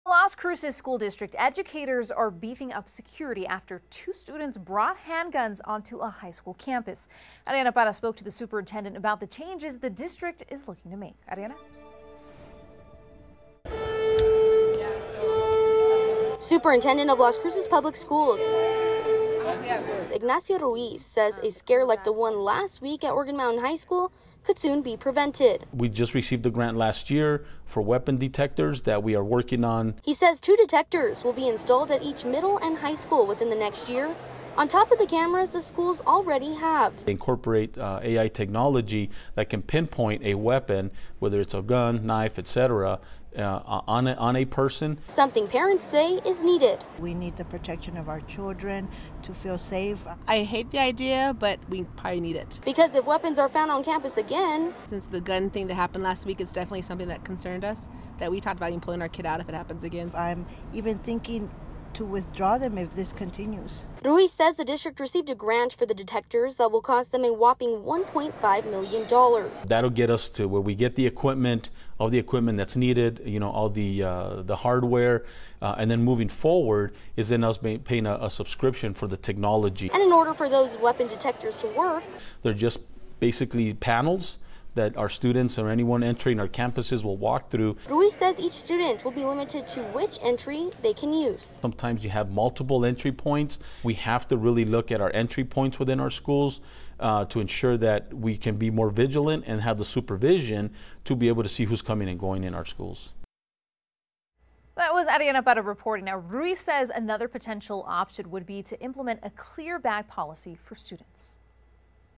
Las Cruces school district to install AI weapon detectors at all middle and high schools
One local school district in the borderland is looking to beef up security, following a recent scare at a high school where two students were allegedly found with handguns on campus.

After the scare, the Las Cruces Public Schools Superintendent said the district is prioritizing the safety of students on campus, with some updated technology.